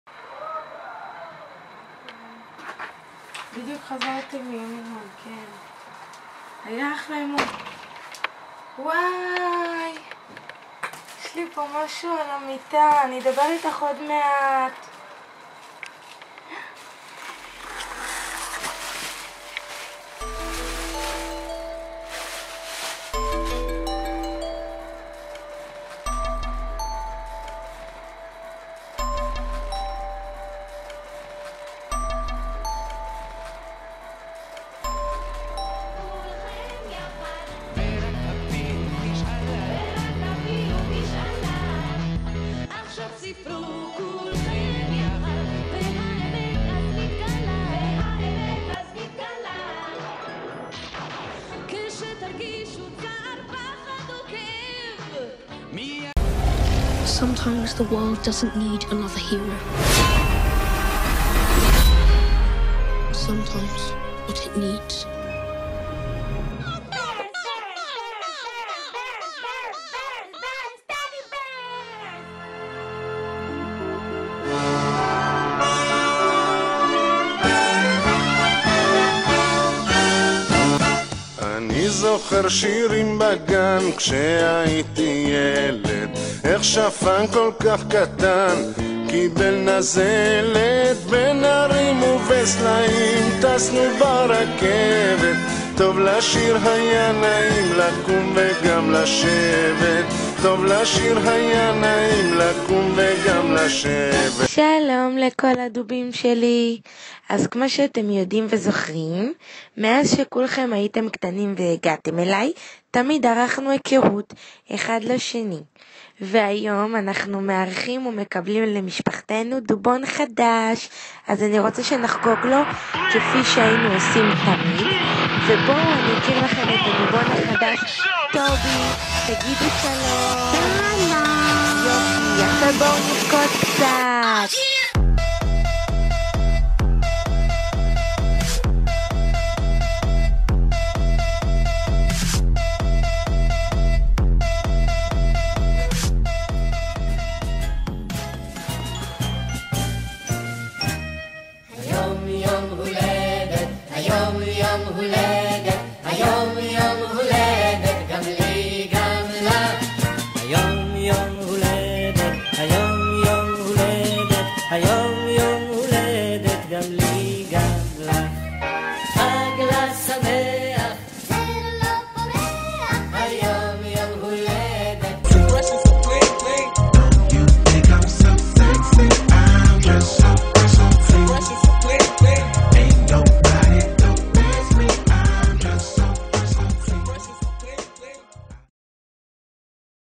Teddy Bear Is All I Care || בובת הדוב לה חבר טוב
סרטון שעשינו אני שחף (הגבר) בוארון וחברתי ליזו רולז על הסיפור חברות ארוך השנים שלה ושל דובים

its a clip about my girlfriend eternal love for teddy bears
since she was a lil baby

00/10/2014